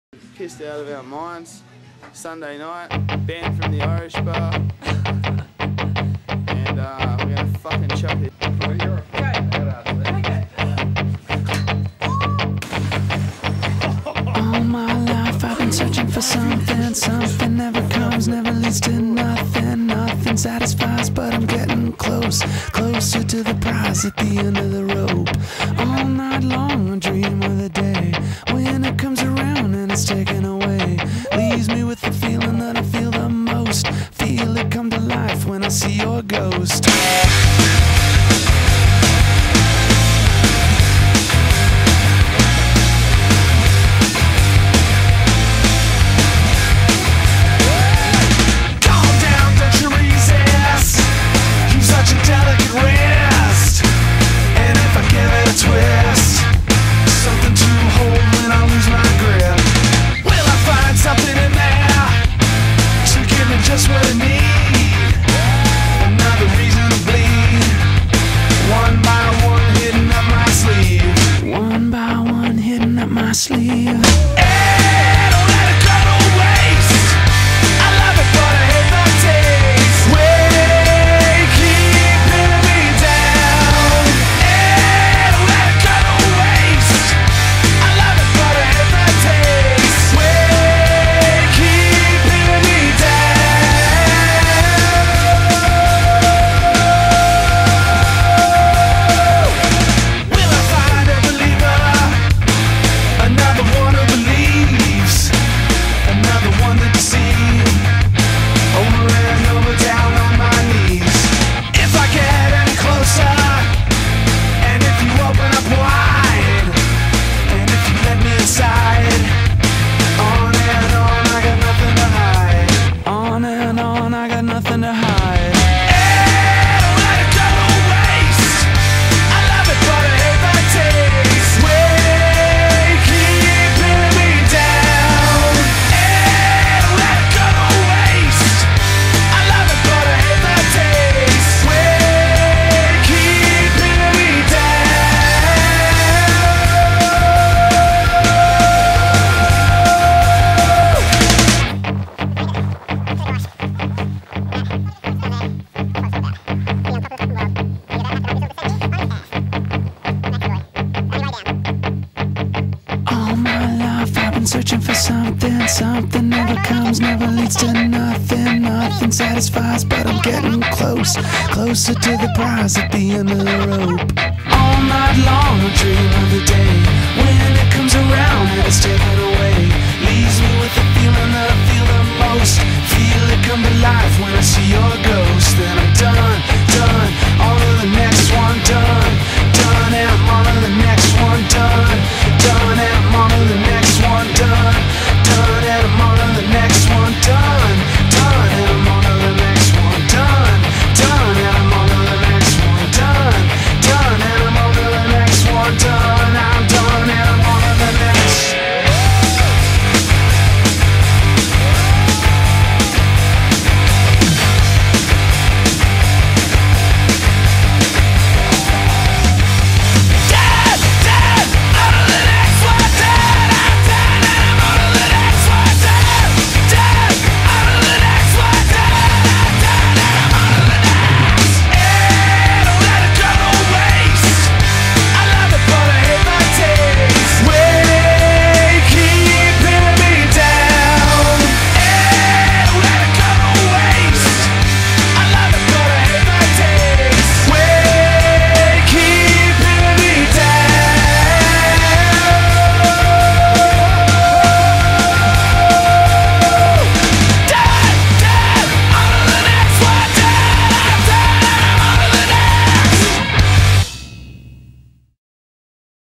The Max Factor
home snowboarding movie filmed in austria 02,03,04,05,06